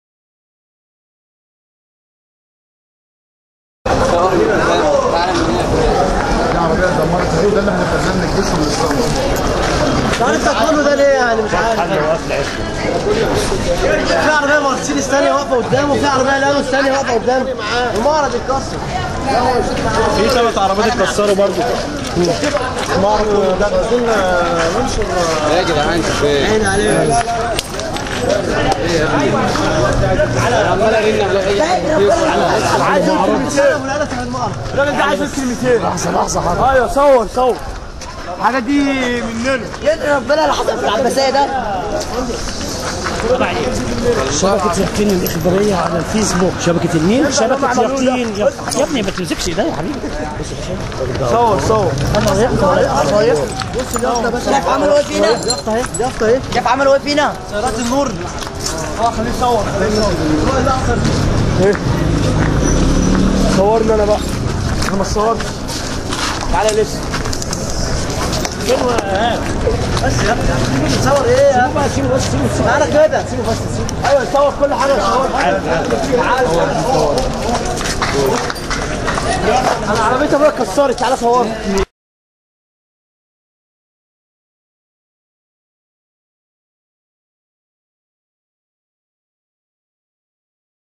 مظاهير و أثر الفوضى والإشتباكات فى العباسية
مظاهير و أثر الفوضى والإشتباكات فى العباسية من قبل البلطجية و الثوار _الجيش فى تمالك تام لأعصابة رغم ما ينهال عليهى من طوب و مولتوف